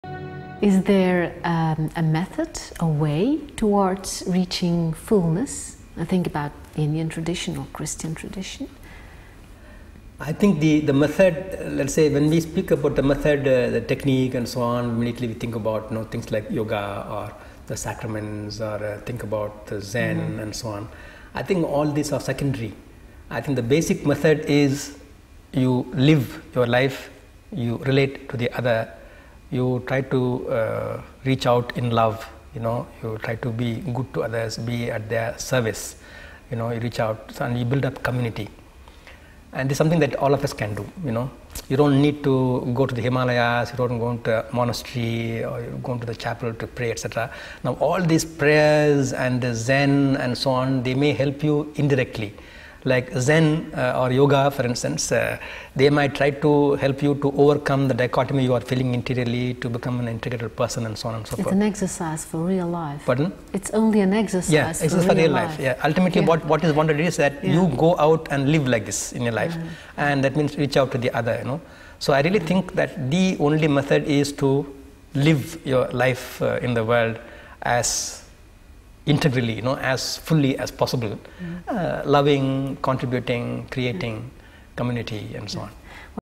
Michael AMALADOSS 'Is there a method to reach 'fullness of being'? Interview Lucette Verboven
Do methods or techniques really work? Is everyday life not the hardest test of all? Listen to what Indian theologian Michael Amaladoss has to say about it in this short interview with Lucette Verboven.

Helpen ze wel: yoga, zen, gebed? Wat is de juiste methode om 'het geluk' waar we allemaal naar streven te bereiken? Luister naar wat theoloog Michael Amaladoss daarover zegt in het korte interview, deel van een groter geheel, met Lucette Verboven.